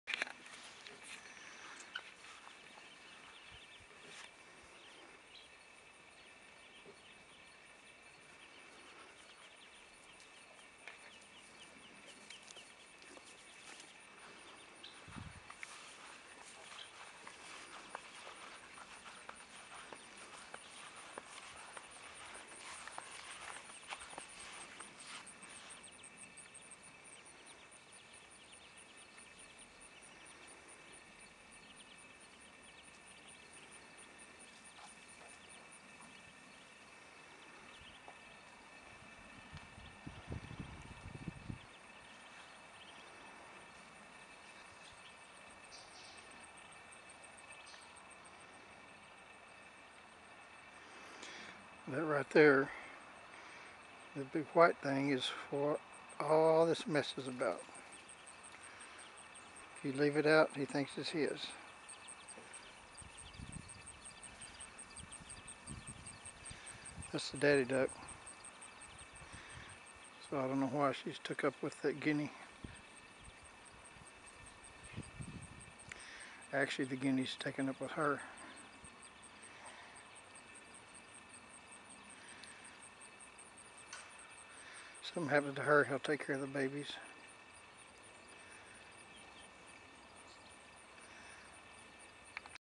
papa Guinea and mother Muscovy
Step father Guinea and mother Muscovy duck are teaching the ducklings to find grains and food. The father, who just adopted the group loves his new family. The original father to the kids has no paternal interest at all and left mother to tend to the entire child rearing process. So the older and nicer Guinea fowl steps in and helps raise the chicks. He even chases other duck suiters away from the mother. This guinea is over 15 years old and that in itself is amazing.